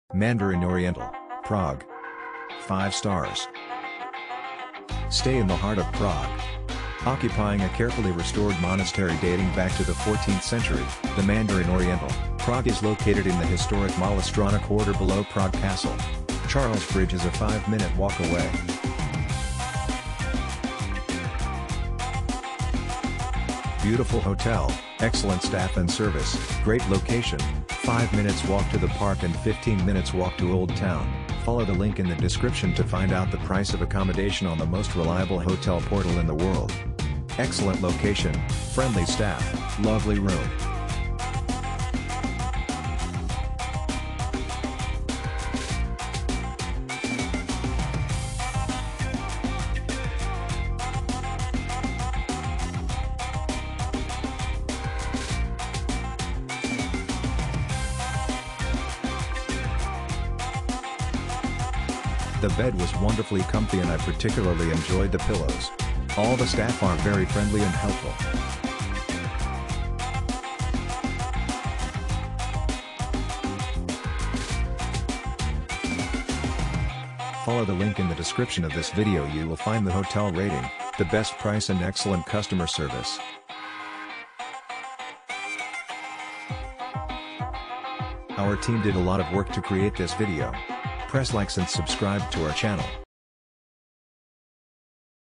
Mandarin Oriental, Prague 5⋆ Review 2019
Mandarin Oriental, Prague 5⋆

Occupying a carefully restored monastery dating back to the 14th century, the Mandarin Oriental, Prague is located in the historic Mala Strana quarter below Prague Castle. Review Mandarin Oriental. Charles Bridge is a 5-minute walk away.  Prague. The rooms at the Prague Mandarin Oriental feature underfloor heating, and guests can choose from various bed linen and pillows. spa Mandarin Oriental. Maid service is offered twice daily, including an evening turn-down.  Prague. Bathroom cosmetics include carefully selected essential oils. resorts Mandarin Oriental. The unique spa is the only one in the world located in a former Renaissance chapel.  Prague. It offers a wide range of spa treatments, yoga classes and a beauty room. travel Mandarin Oriental. Spices Restaurant and Bar invites guests to a world of authentic Asian flavors and aromas.  Prague. Asian fares along with original cocktails, wine and tea can be served either at the dining table in the restaurant, at the bar or in a private booth. luxury Mandarin Oriental. Spices Lounge and a covered outdoor terrace offer light relaxation and refreshment such as salads, traditional Asian bento boxes as well as classic beef burgers.  Prague. The Hellichova Tram Stop is 350 feet away from the Mandarin Prague. Hotel Prague.
Prague 01 is a great choice for travelers interested in history, architecture and atmosphere.
 . Review Prague.